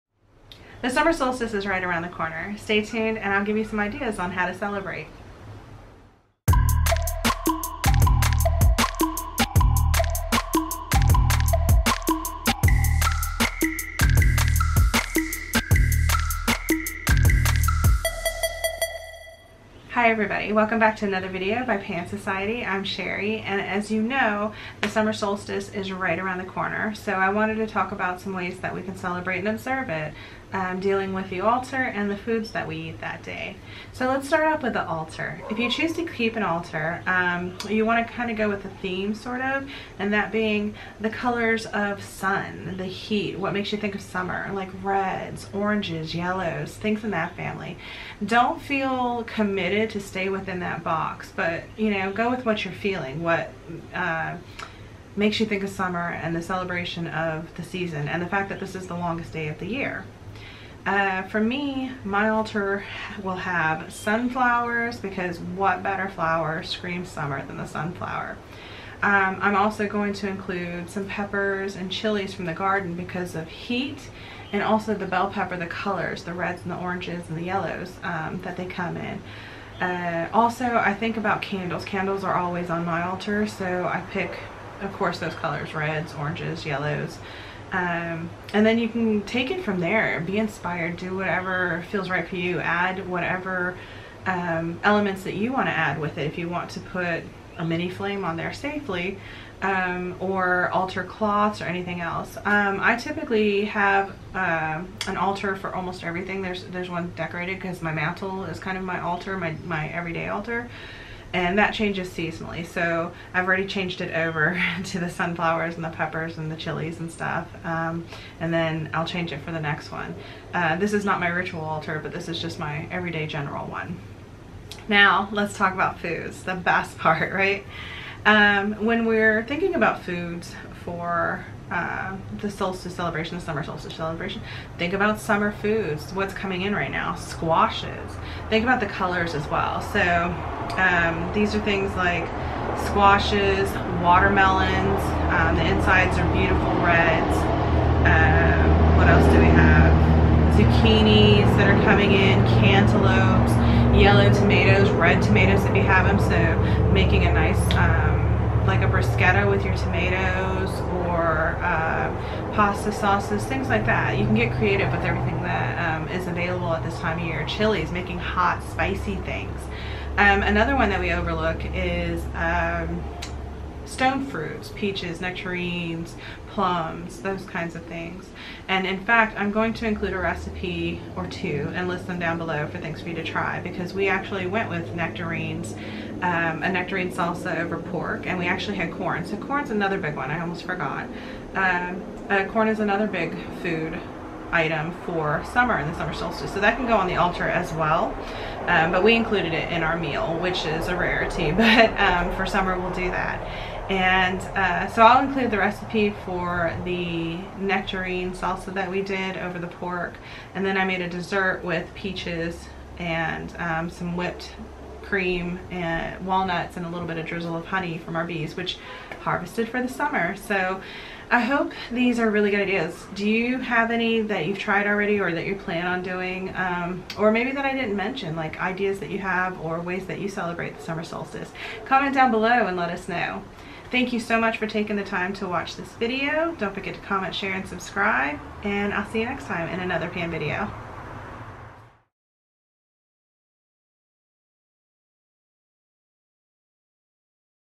Summer Solstice Altar and Food | Cherie Calvert, Pan Society
Here are some ideas for your summer solstice altar and  summer solstice food.

Cherie is a co-founder of Pan Society. She grew up with animism and is an eclectic practitioner. Cherie is a clinical herbalist and loves being able to share nature's best creations with others.